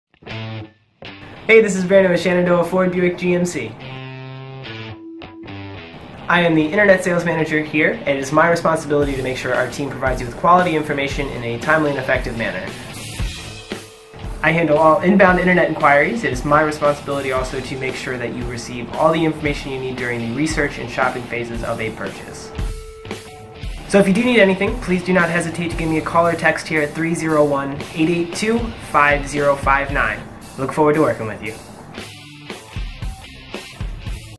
Brandon Harmon - Internet Sales Manager | Front Royal, VA
Brandon Harmon is our Internet Sales Manager at Shenandoah Ford Buick and GMC. Come out